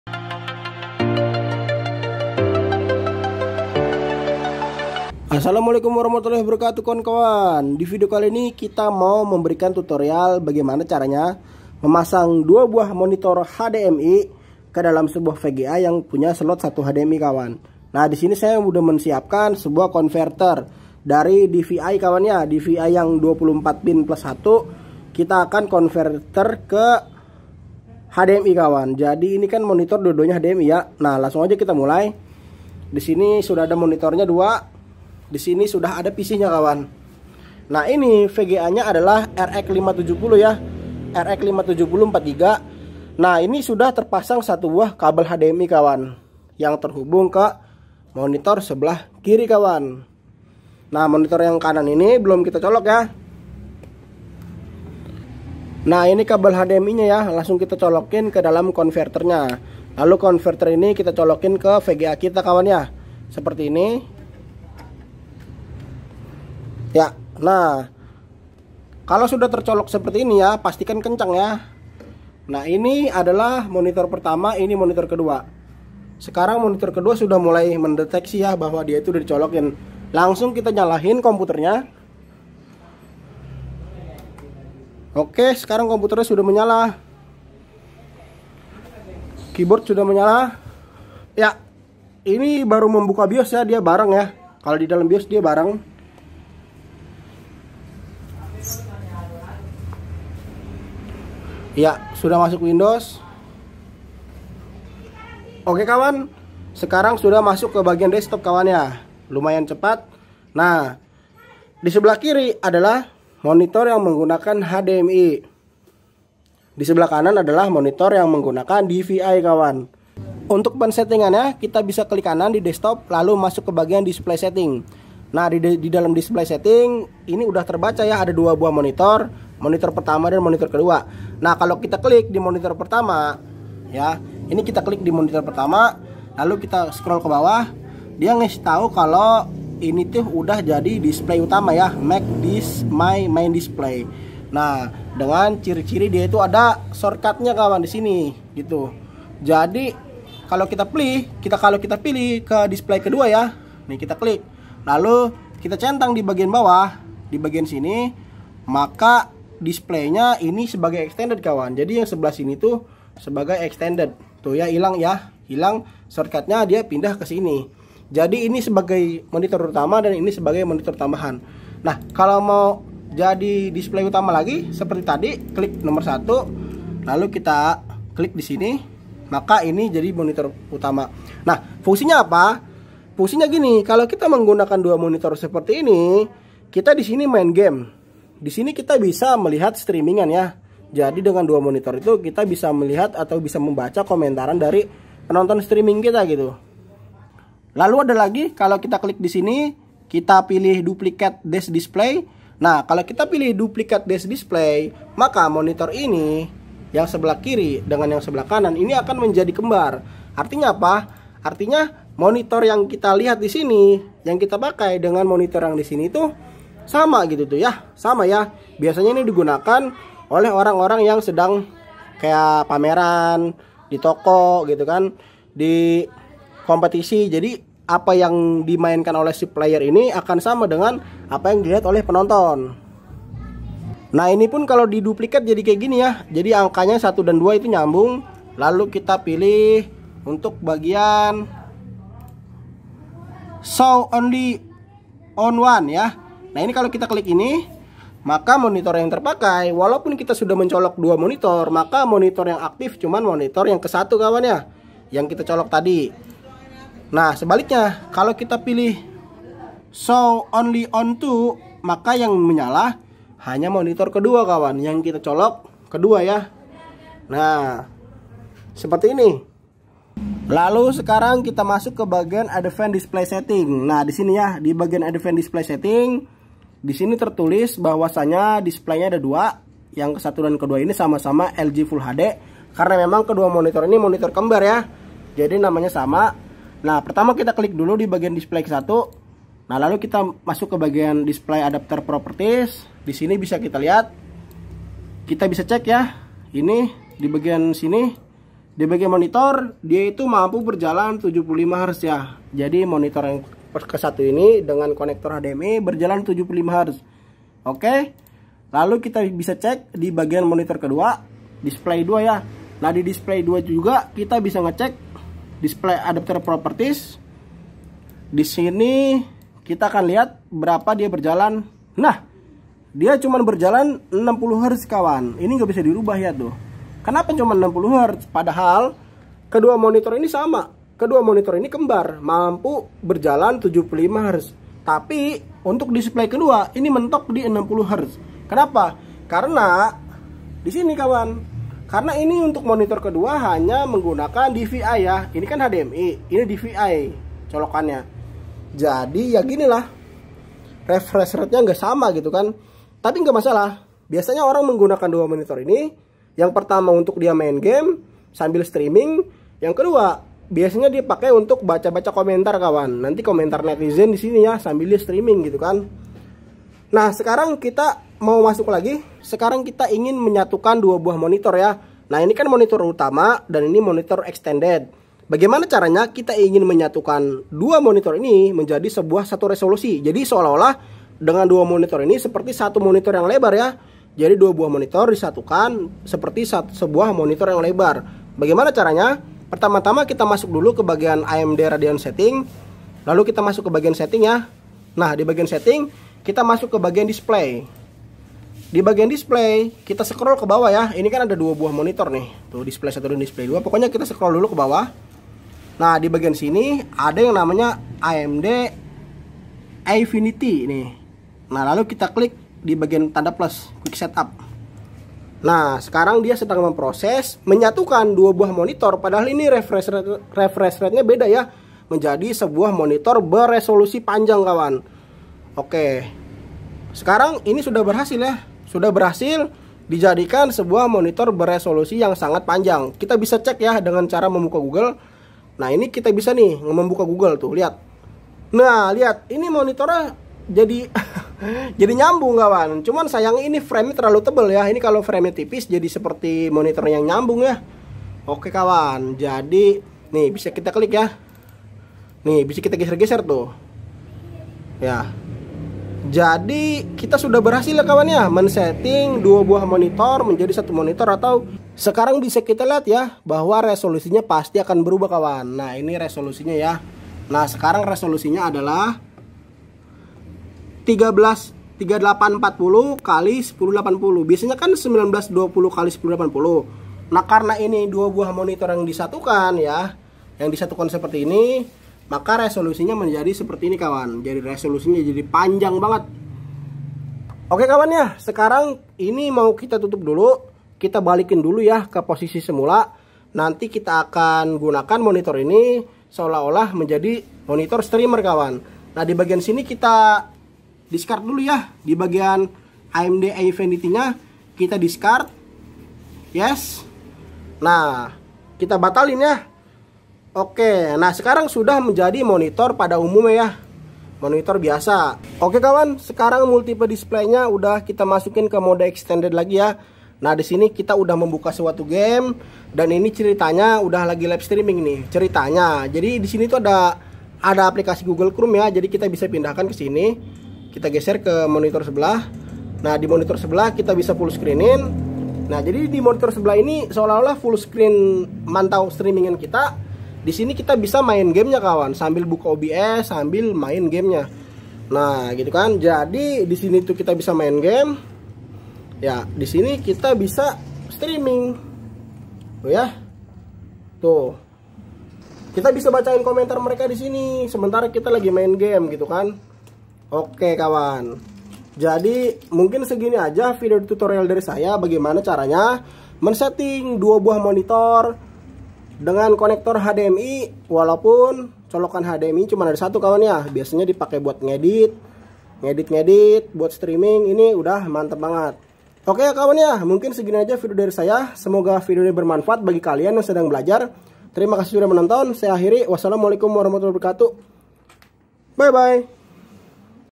Cara Dual Monitor Untuk 1 PC Streamer Editor di Windows 10
Cara menggunakan dua monitor untuk satu komputer sudah saya jelaskan di video tersebut hanya saja saya menggunakan 2 kabel HDMI yang mana satu lagi saya pakai konverter DVI-D 24+1 pin untuk HDMI monitor ke dua,
adapun fungsi dual monitor sangatlah banyak, bisa untuk melihat komentar penonton saat streaming bisa juga untuk multi tasking saat EDITING sehingga memudahkan kita untuk pekerjaan kita dan lain lain...

Semoga video ini bermanfaat bagi kita semua
Komentar jika ada yang ingin ditanyakan